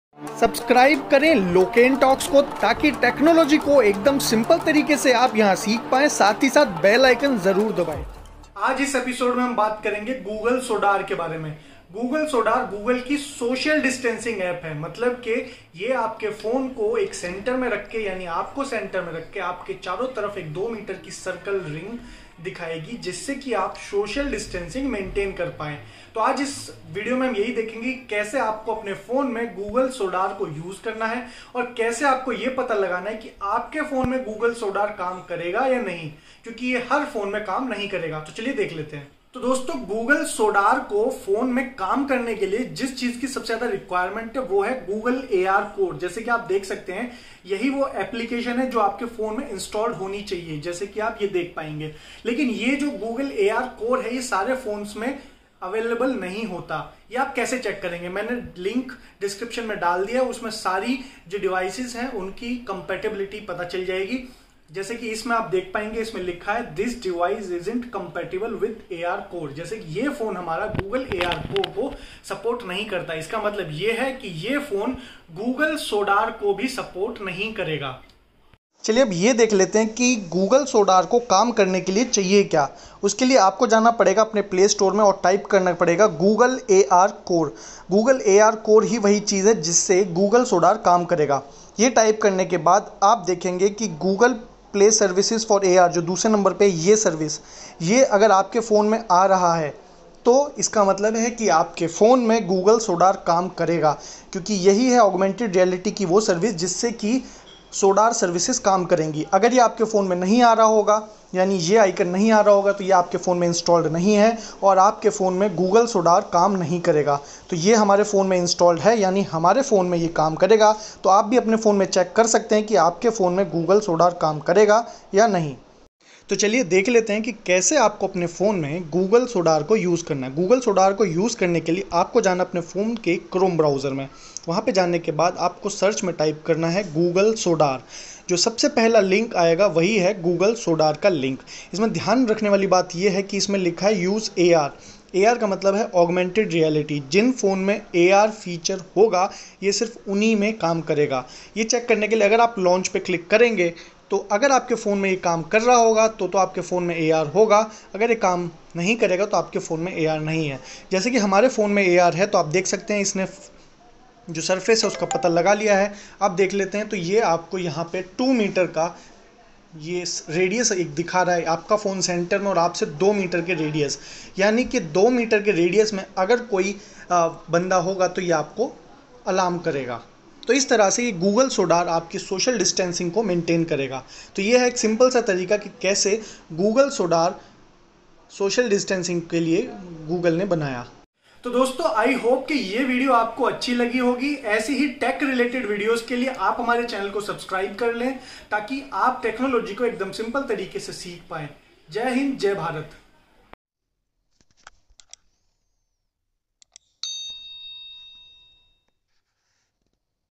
Google Sodar | New Social distancing App | Check Your phone's Compatibility
This is the video tutorial on how to use google sodar app. Google sodar app is the latest AR app by Google for social distancing.
So Sodar app is also known as Social distancing app.
To use Google Sodar App your Phone must support AR services by Google otherwise Google Sodar App will not work on yourPhone.

To check your phone's Compatibility with Google AR core